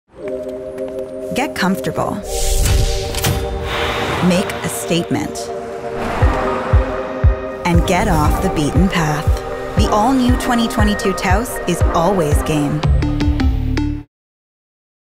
Make a Statement in the 2022 Taos.
Make a statement in the all-new 2022 Taos. Our small(ish) SUV is in stock today and has the space, style, and driving thrills for all your big real-life adventures. The Taos boasts a responsive turbocharged engine, impressive cargo space, and standout design.